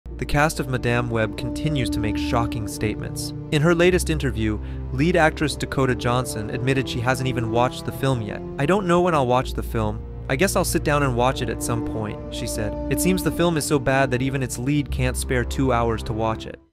The WORST Statement an ACTOR can ever make
Marvel's new Fantastic Four team has finally been officially announced. We'll see Vanessa Kirby, who has recently gained significant popularity, taking on the role of Sue Storm. Joseph Quinn, known from "Stranger Things," will play her brother, Johnny Storm. Ebon Moss-Bachrach, recognizable from "The Bear," will portray The Thing. The role of Reed Richards had already been revealed some time ago, with Pedro Pascal, famous for his roles in "The Mandalorian" and "The Last of Us," playing Mr. Fantastic. The film's release date has also been officially announced: "Fantastic Four" will hit theaters on July 25, 2025.